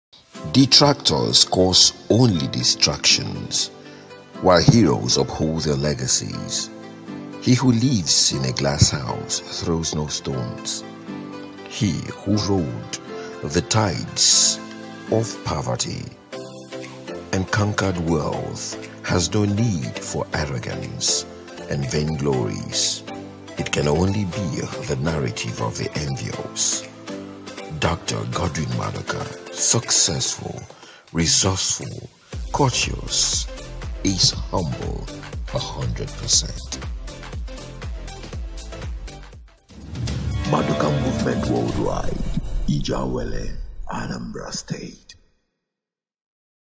Dr. Godwin Maduka upholds his Legacy in Anambra State
his legacy projects in Anambra State have given him the advantage ahead of others in Anambra State Governorship race. he is an outstanding personality that has gone beyond philanthropism, but his projects places him as an executive Governor